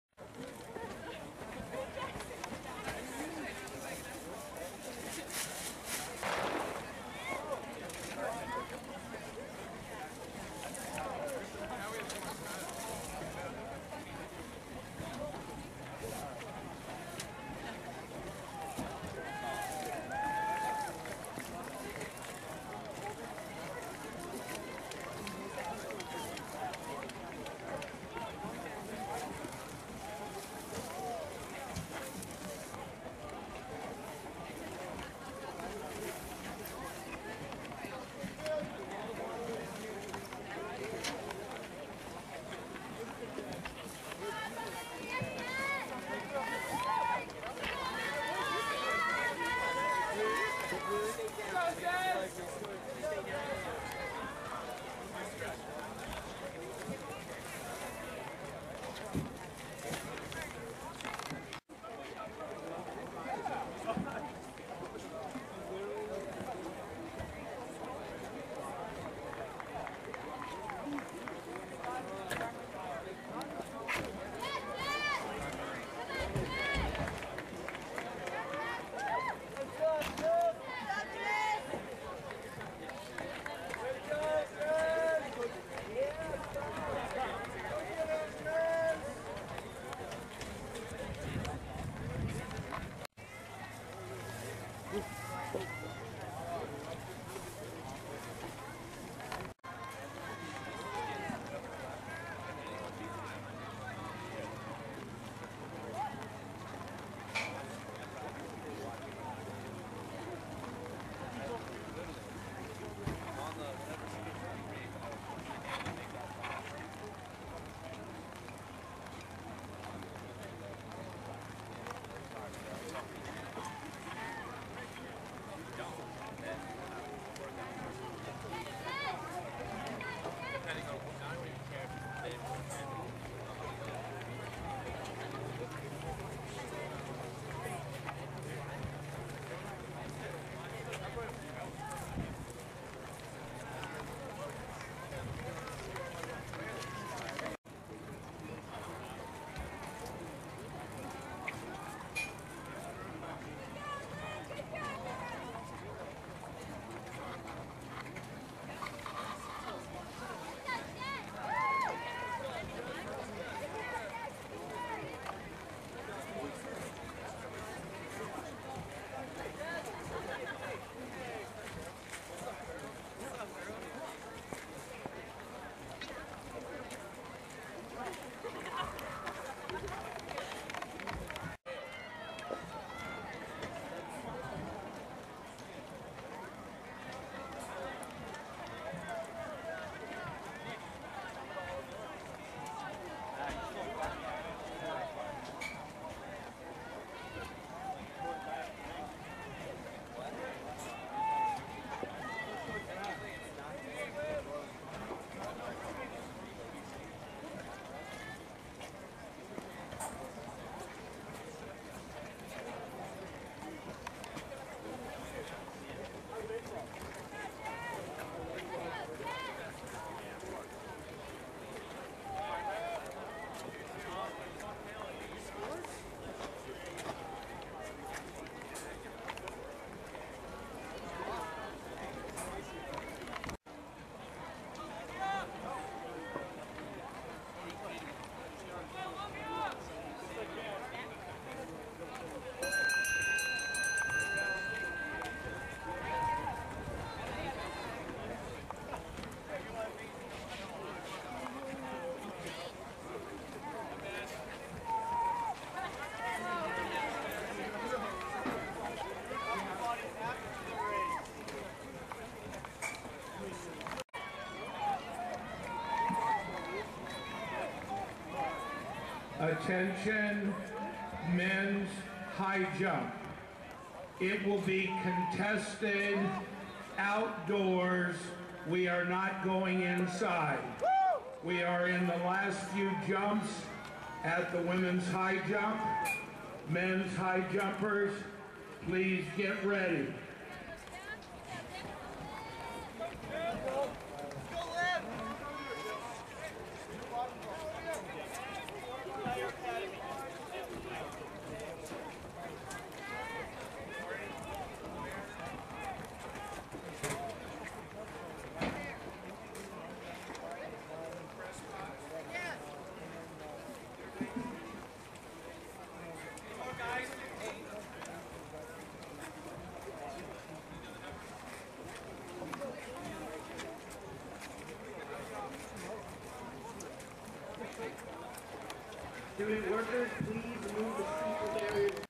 2014 CGA INVITATIONAL WOMENS 3K STEEPLECHASE
My apologies for not being able to post the Men's 3K steeplechase due to a memory malfunction.
Here are the women's results:

Women     3000     Meter     Steeplechase
Name     Year     School     Finals     Points
1     Winter,     Emily     Conn     College     12:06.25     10
2     Marshall,     Olivia     Westfield     St     12:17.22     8
3     Mulroy,     Jessica     Coast     Guard     12:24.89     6